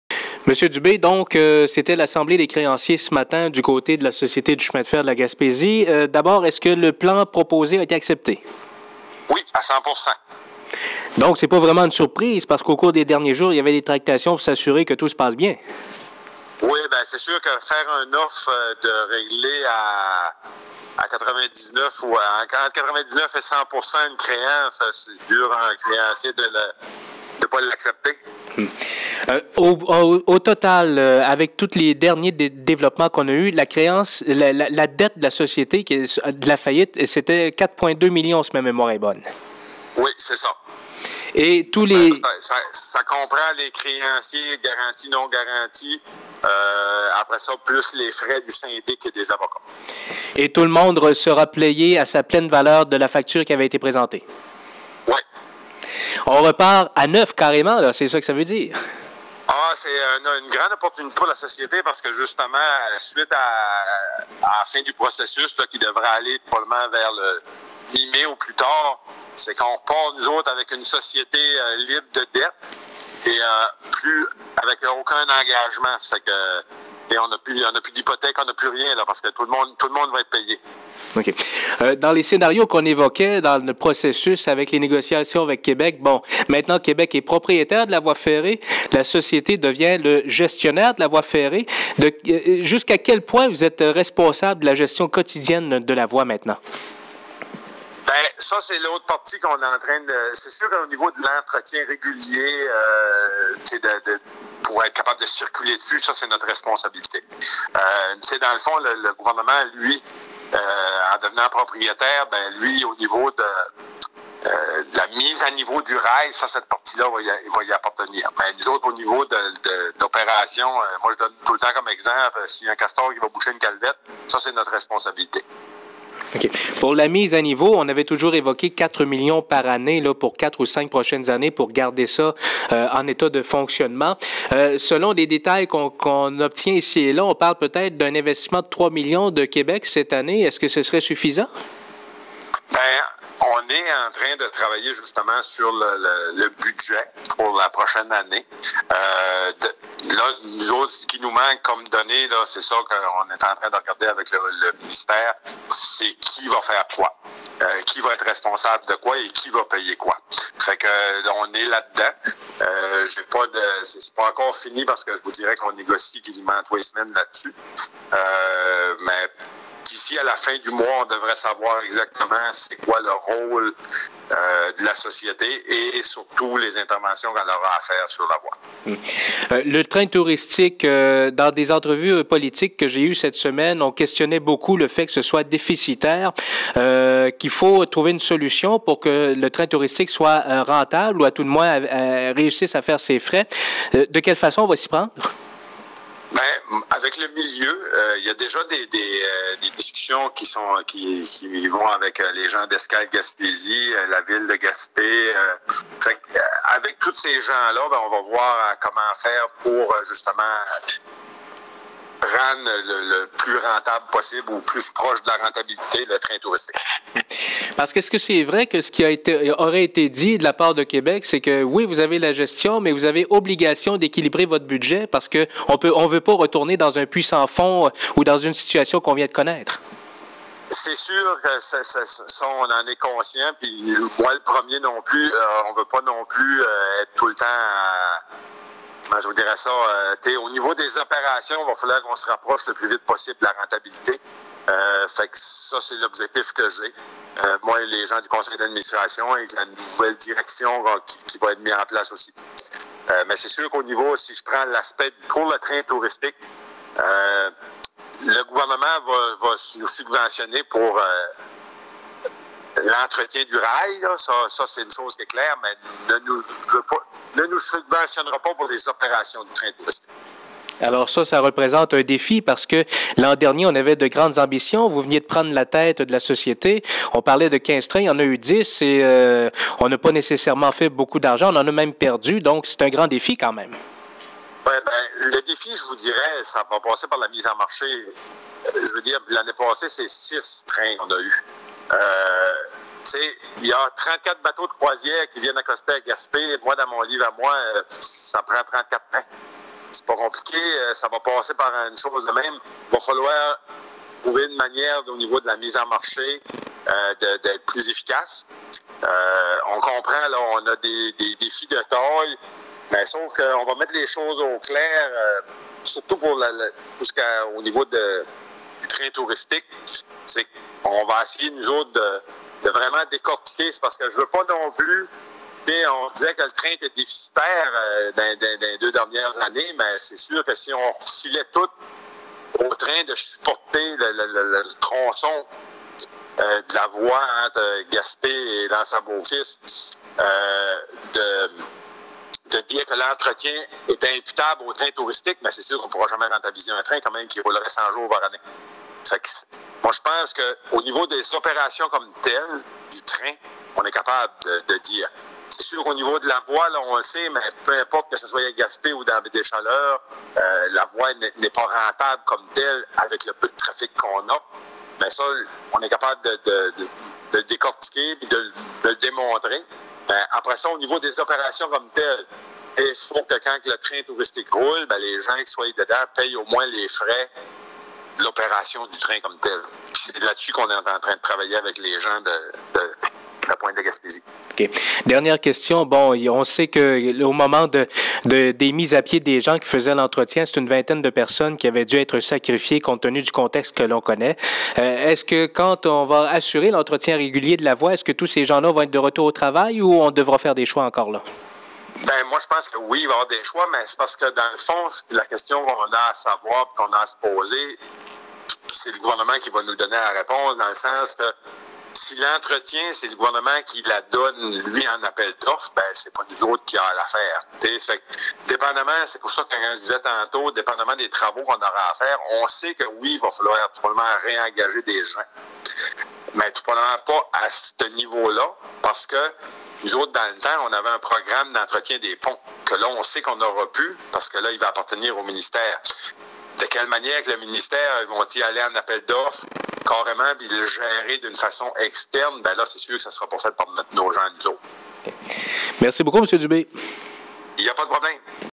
Entrevue Éric Dubé, Société du chemin de fer de la Gaspésie - 9 avril 2015
Les créanciers ont accepté l'offre de la Société du chemin de fer de la Gaspésie. Le président, Éric Dubé, donne les détails à Nelson Sergerie.